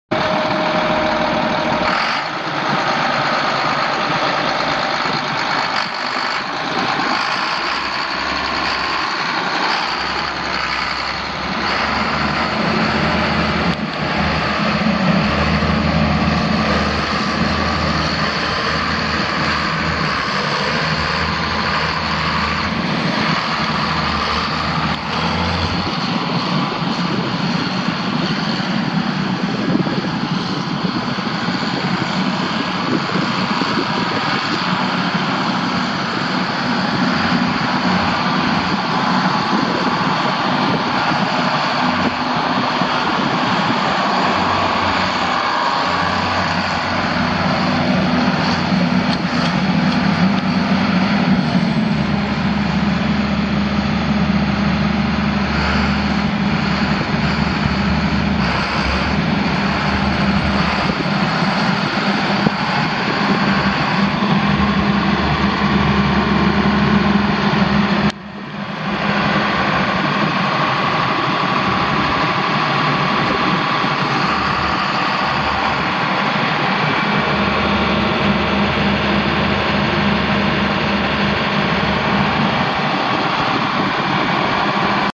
1988 John Deere 4850 MFWD tractor for sale at auction | bidding closes August 14, 2019
1988 John Deere 4850 MFWD tractor
785 hours on meter
John Deere 6466AR-17 six cylinder diesel engine
Serial RG6466A338883
Powershift transmission
15F-4R gears
AC and heat
AM/FM
Three point
Top link
Quick attach
18.4-26 front tires
20.8R42 dual rear tires
AC does not blow cold
Serial RW4850P013131
It's located in Dighton, KS, and we're selling it to the highest bidder regardless of price.